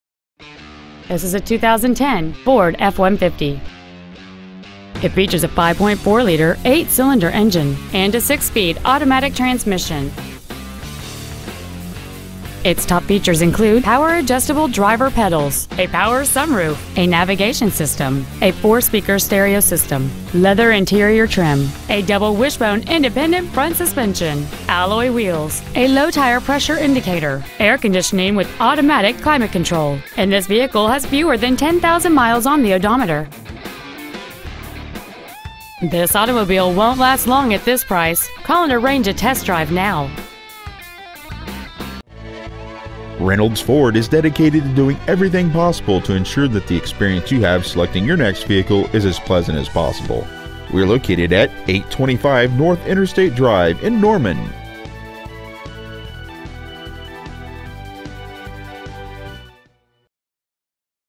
2010 FORD F-150 Norman OK
Year : 2010
 Make : FORD
 Model : F-150
 Engine : 5.4L V8
 Trans . : AUTO 6SPD
 Exterior : WHITE
 Miles : 9,710
 Interior : GRAY
 Stock : FB172A